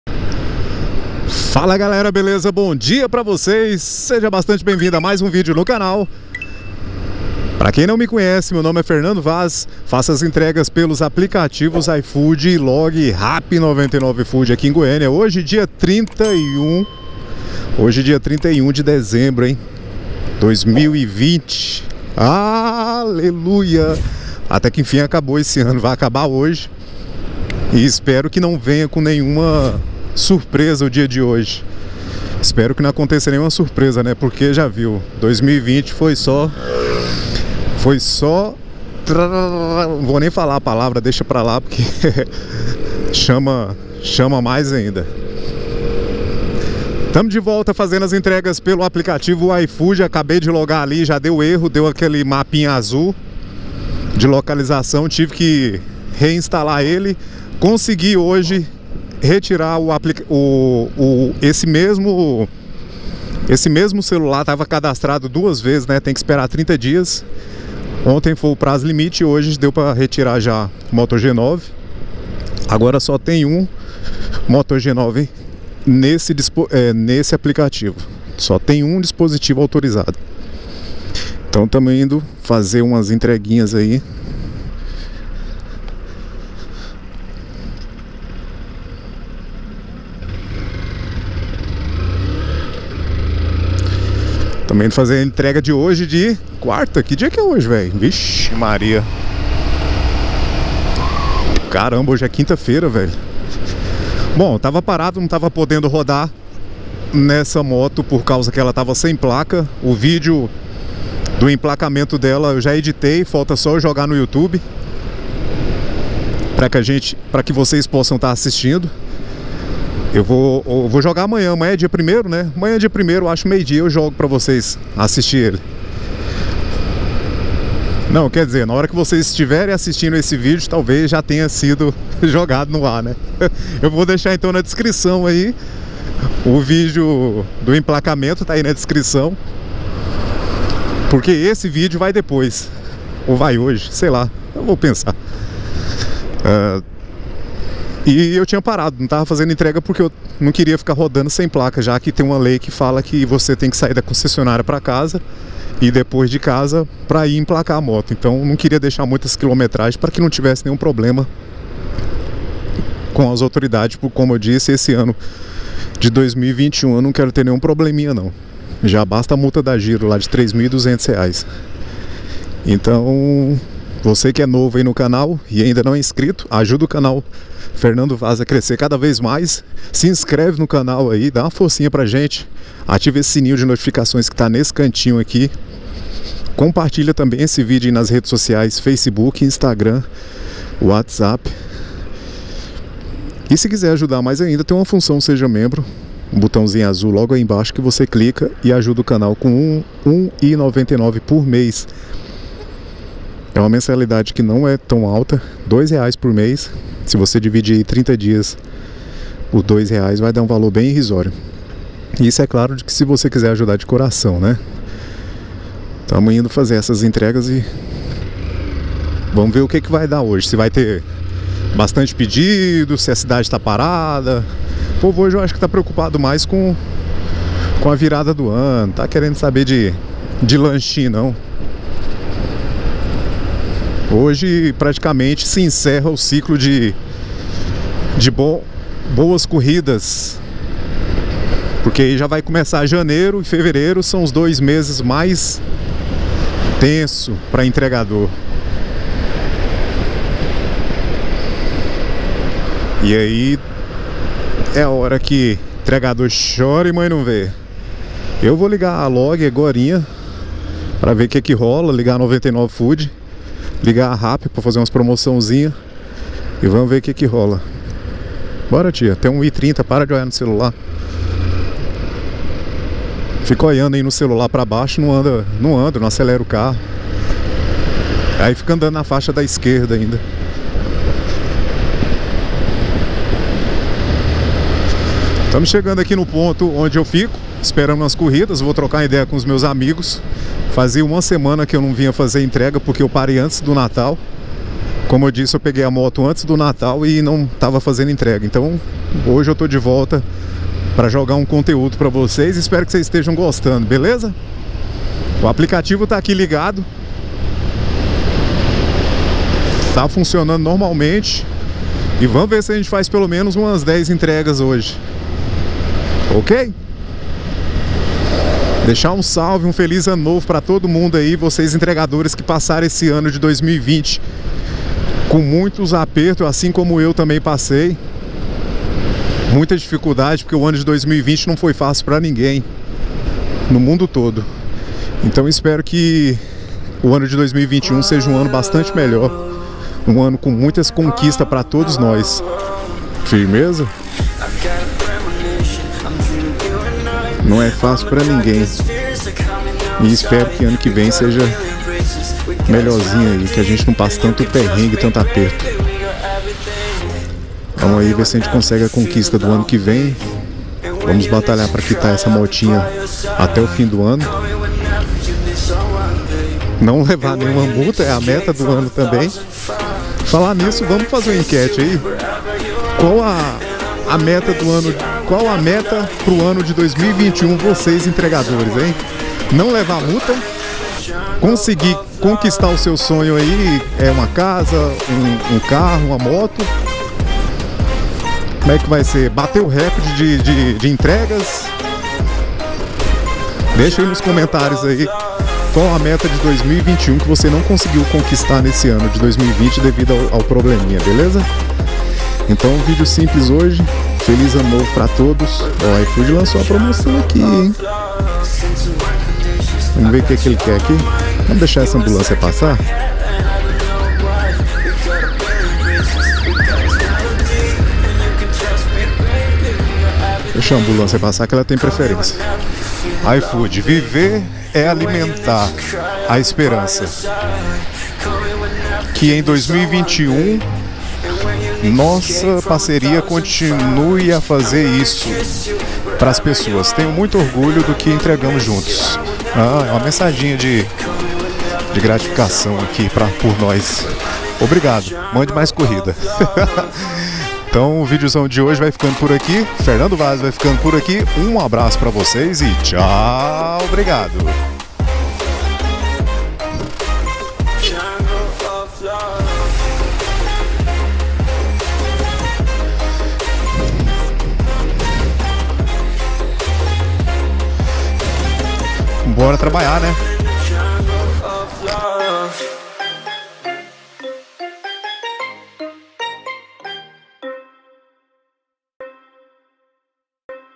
IFOOD ENTREGADOR ÚLTIMO MOTOVLOG DO ANO DE 2020 QUAL SUA META PARA O ANO DE 2021 ENTREGADOR?
Equipamentos Para gravação
Camera=Gopro hero5 black
Microfone de Lapela= Dudacell
Adaptador De Microfone Original da Gopro
Caixa case= Gopro

créditos a música no final do vídeo ao canal NCS